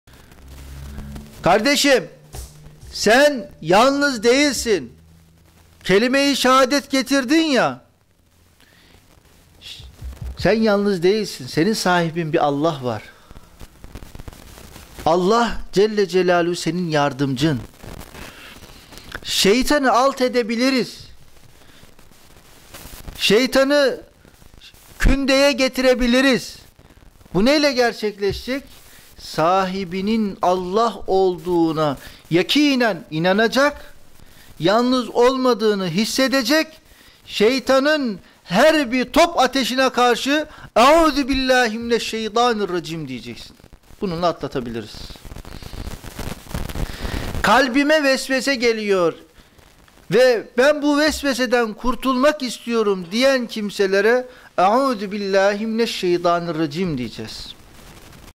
Şeytanı Kündeye Getirebiliriz - Talha YİĞİT - Haramlarla Mücadele Derneği
Talha YİĞİT Hocamızın 26 Kasım 2013 tarihinde Haramlarla Mücadele Derneği teşkilat mensuplarına verdiği " Mayına Dikkat " isimli dersinden bir bölümü istifadenize sunuyoruz.